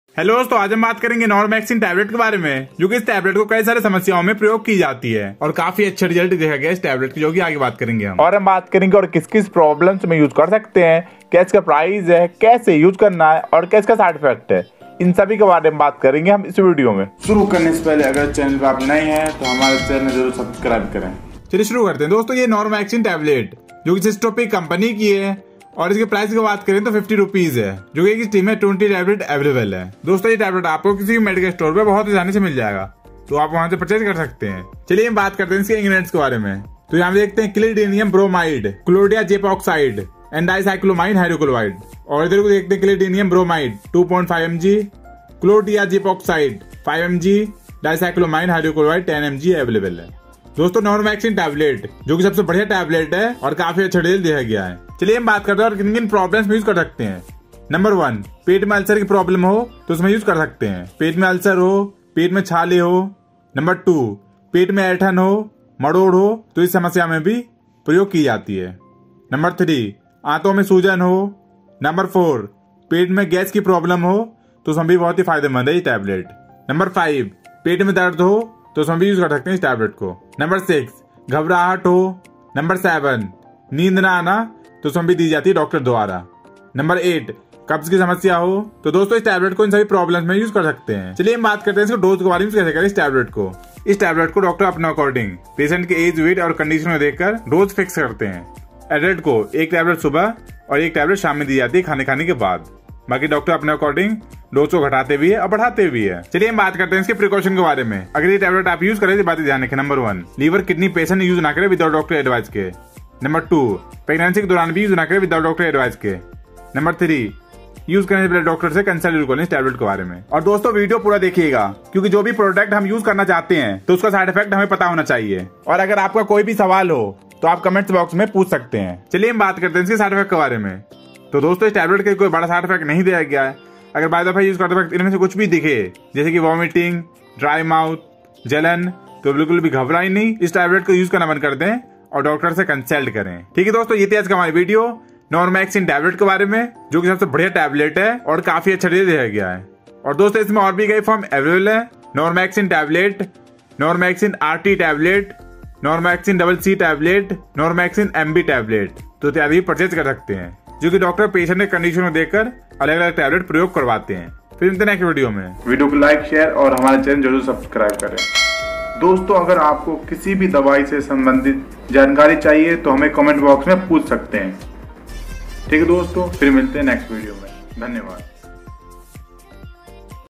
normaxin tablet | कब कितना और कैसे लें | normaxin tablet hindi | normaxin rt capsule uses in hindi
Hello Guys
Aaj Ki Video Me Ham Baat karenge Normaxin Tablet Ke Baare Me Ki Ye Kis Kis Problems Me Use Kar Sakte Hai Aur Baat Karenge
2. Eske Benifits kiya Kiya Hai
3. Use Kaise Karna Hai
4. Kiya Eske Side Effects Hai

Let's start """"""""

Notice